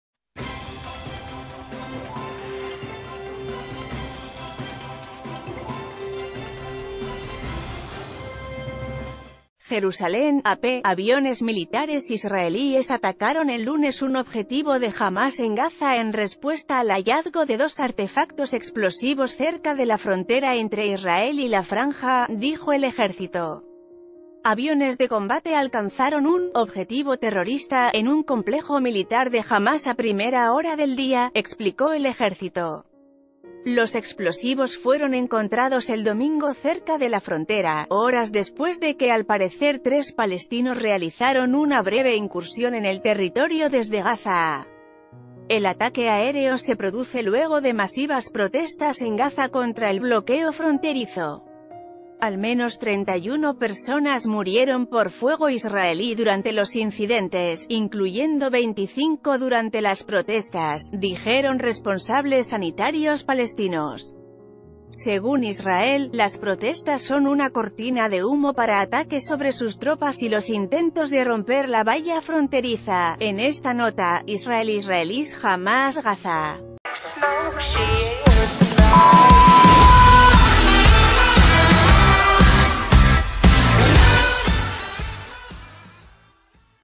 Israel ataca Gaza tras hallar explosivos en la frontera
JERUSALÉN (AP) — Aviones militares israelíes atacaron el lunes un objetivo de Hamas en Gaza en respuesta al hallazgo de dos artefactos explosivos cerca de la frontera entre Israel y la Franja, dijo el ejército. Aviones de combate alcanzaron un “objetivo terrorista” en un complejo militar de Hamas a ...